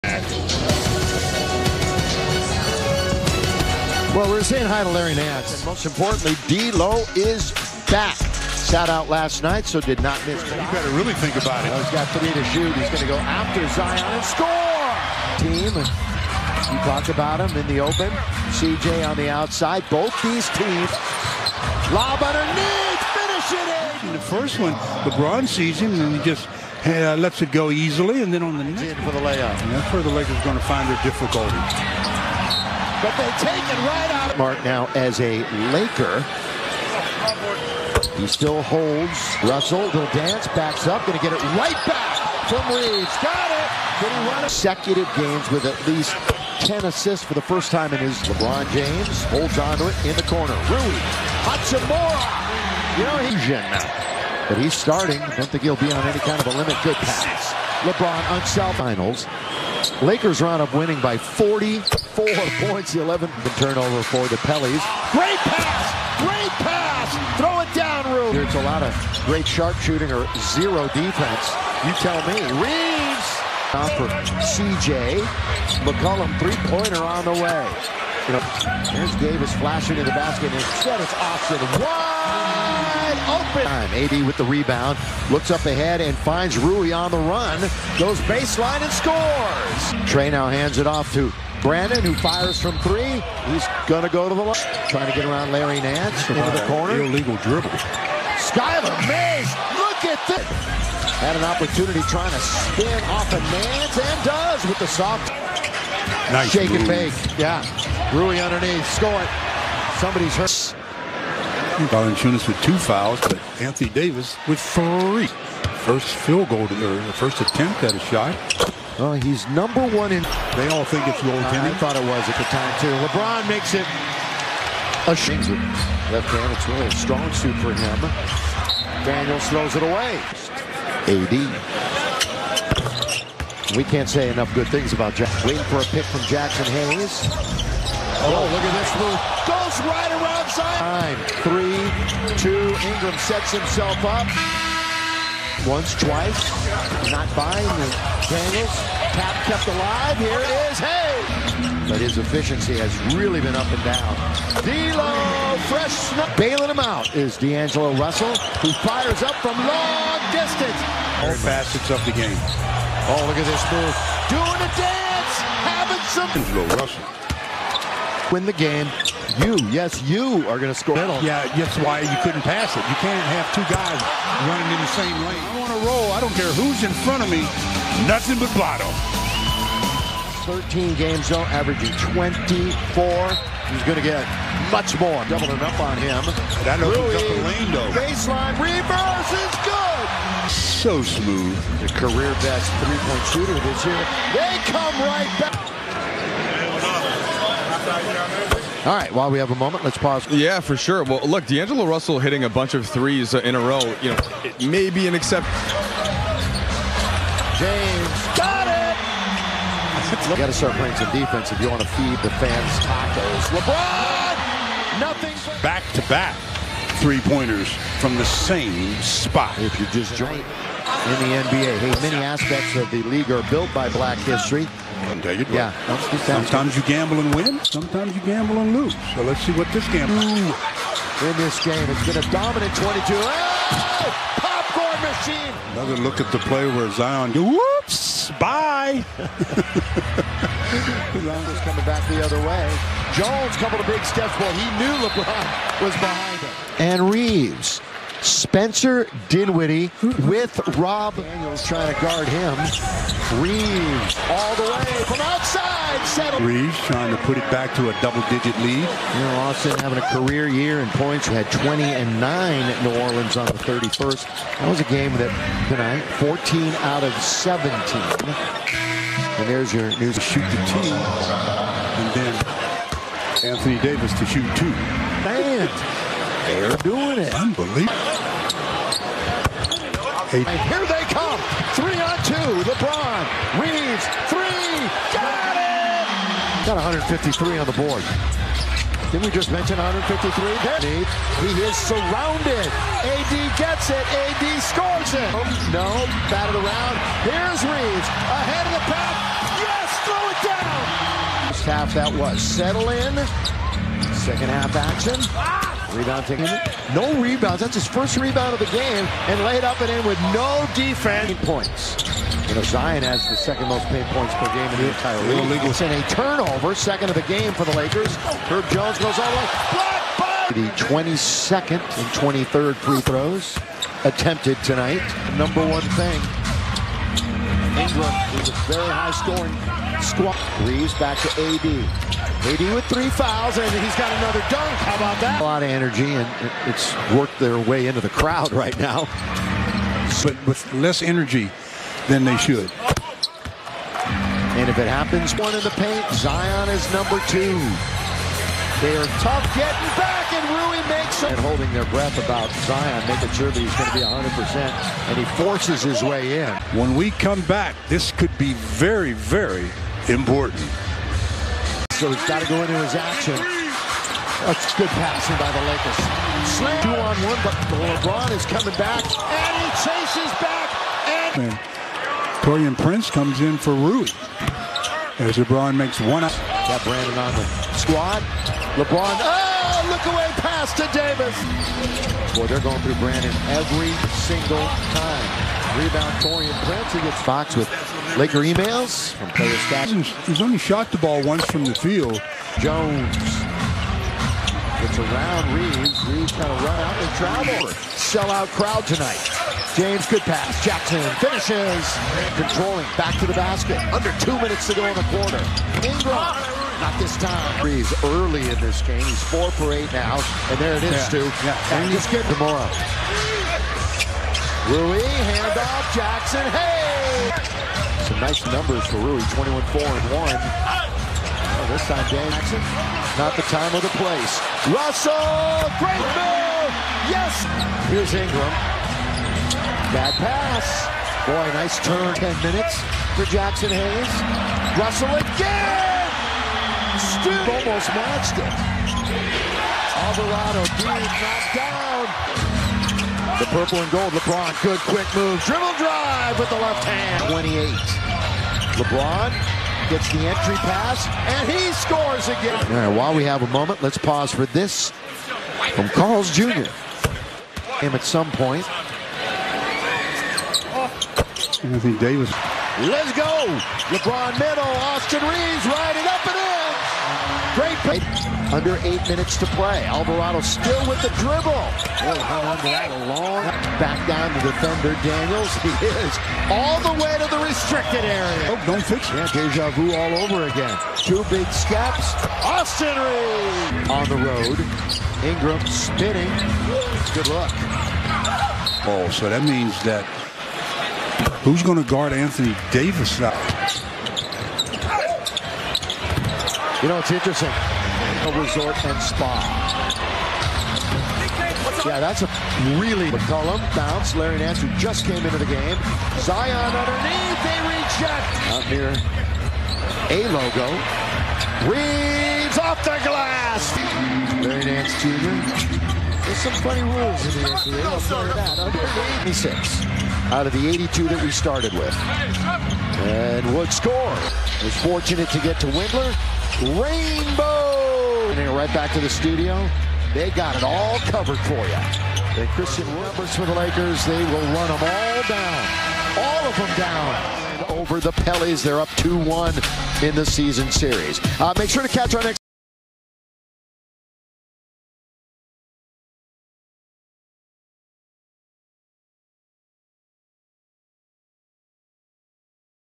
Lakers vs Pelicans | Lakers GameTimeTV | Lakers Team Highlights
Lakers bounce back nicely after yesterday's loss to the Nuggets!
It was nice to have D'lo back. Lakers played a great game make 45% of their 3s and basically getting anything they wanted. Now 3 days off until their next game vs the Pistons on Tuesday! Lets keeping it Rolling.

Also, what yall think Spencer Dinwiddie was doing at the game??